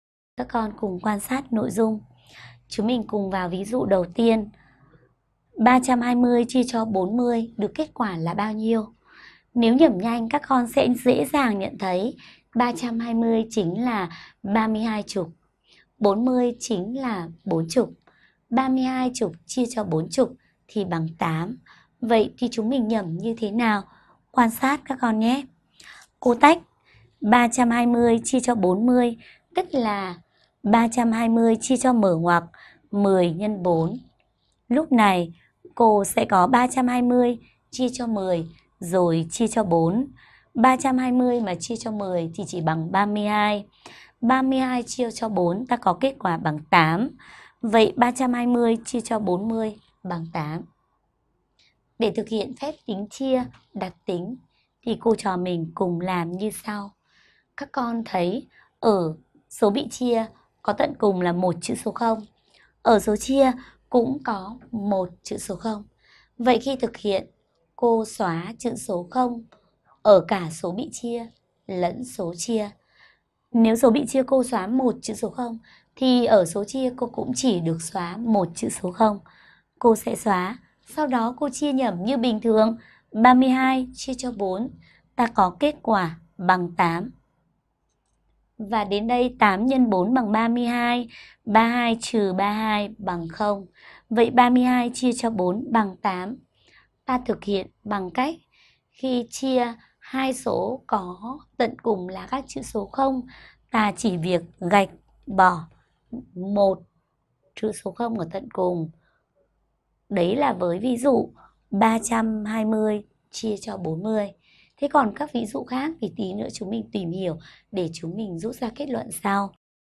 Chia hai số có tận cùng là các chữ số 0 - Toán 4
Học trực tuyến Toán 4
a) 320: 40 = ?
320: 40 = 320 : (10 x 4)
             = 320 : 10 : 4
             = 32 : 4
             = 8
320: 40 = 8
Nhận xét: 320 : 40 = 32 : 4
Khi thực hiện phép chia 320 : 40, ta có thể cùng xoá một chữ số 0 ở tận cùng của số chia và số bị chia, rồi chia như thường.
32000: 400 = 32000 : (100 x 4)
                   = 32000 : 100 : 4
                   = 320 : 4
                   = 80
32000: 400 = 80
Nhận xét: 32000 : 400 = 320 : 4
Khi thực hiện phép chia 32000 : 400, ta có thể cùng xoá hai chữ số 0 ở tận cùng của số chia và số bị chia, rồi chia như thường.
c) Khi thực hiện hai số có tận cùng là các chữ số 0, ta có thể cùng xoá một, hai, ba, … chữ số 0 ở tận cùng của số chia và số bị chia, rồi chia như thường.
Xem thêm bài giảng
CHIA HAI SỐ CÓ TẬN CÙNG LÀ CÁC CHỮ SỐ 0